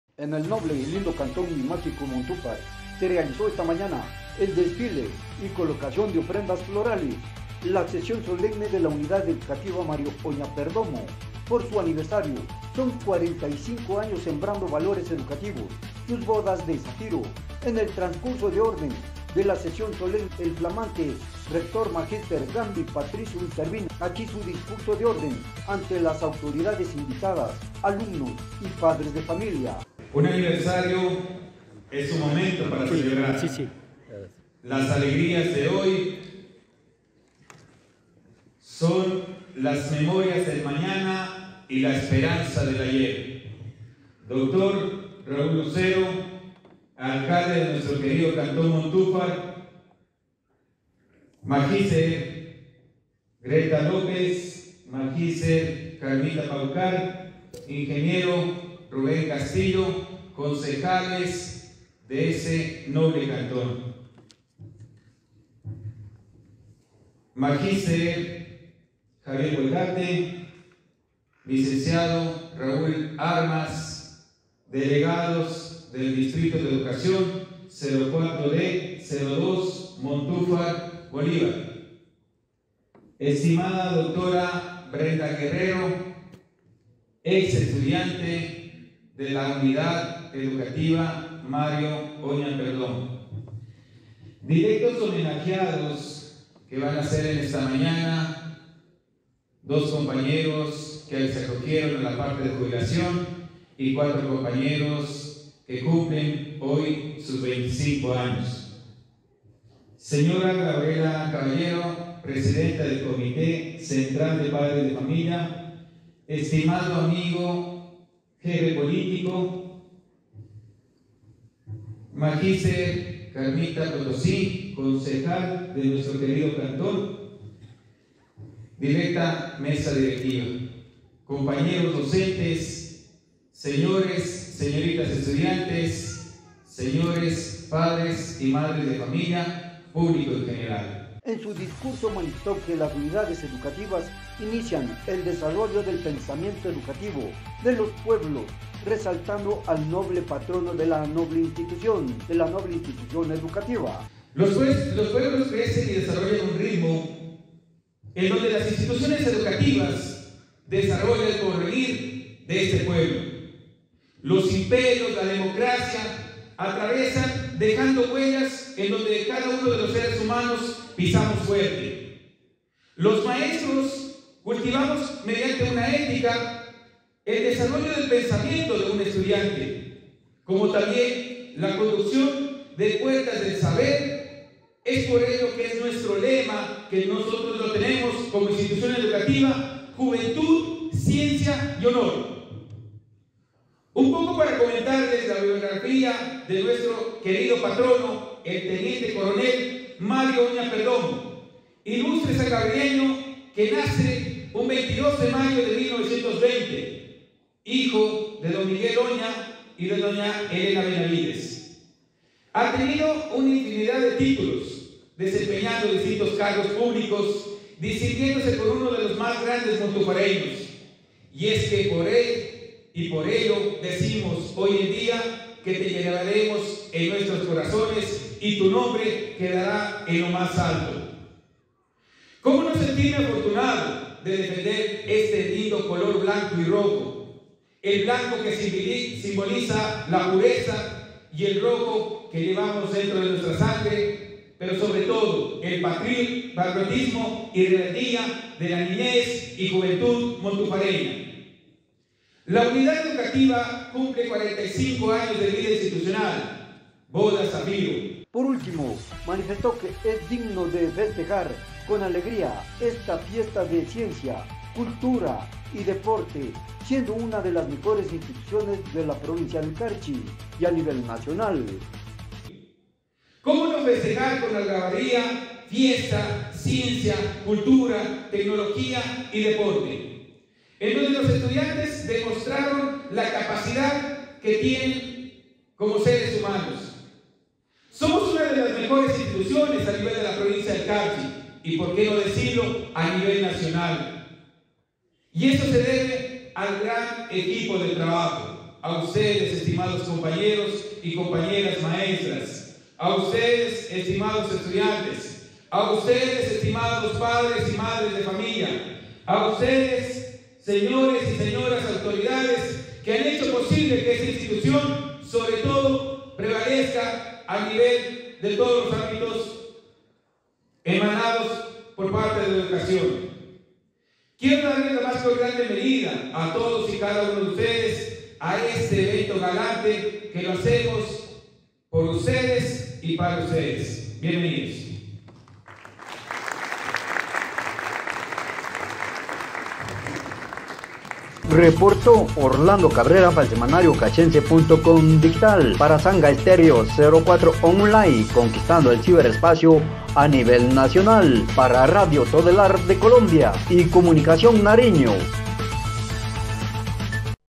UNIDAD EDUCATIVA MARIO OÑA PERDOMO DE ANIVERSARIO 45 AÑOS BODAS DE ZAFIRO
Semanario “Carchense”. Com. Digital.
En el noble y lindo cantón mágico Montufar se realizó esta mañana el desfile, colocación de ofrendas florales, la sesión solemne de la Unidad Educativa Mario Oña Perdomo por su aniversario son 45 Años sembrando valores educativos sus bodas de Zafiro. En el trascurso de orden de la Sesión solemne el flaméate rector el Mgtr. Gandi Patricio Intervino aquí su discurso de orden ante las autoridades invitadas alumnos y padres de familia.
En su discurso manifestó que las unidades educativas inician el desarrollo del pensamiento educativo de los pueblos resaltando al noble patrono de la Institución.
Por último, manifestó que es digno de festejar con algarabía esta fiesta de ciencia, cultura y deporte, siendo una de las mejores instituciones del Carchi a nivel nacional.